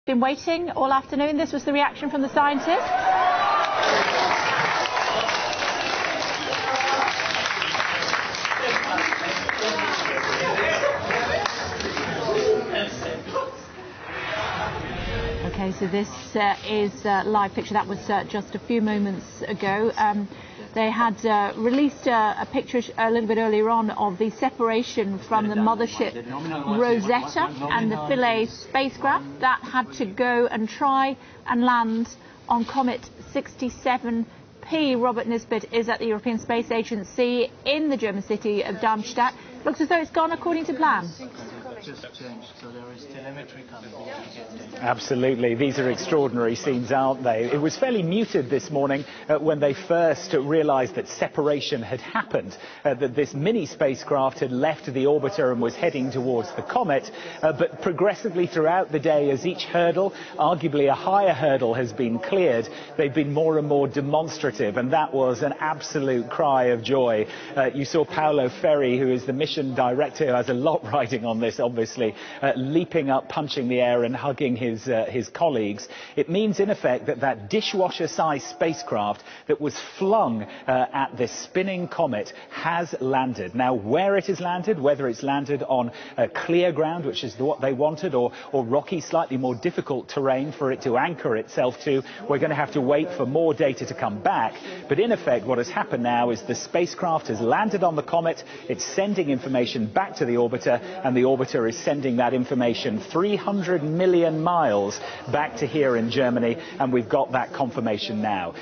The Moment The Rosetta Probe Landed On Comet
Watch scientists at the European Space Agency celebrate as they confirm the Philea probe has landed successfully has landed on Comet 67p.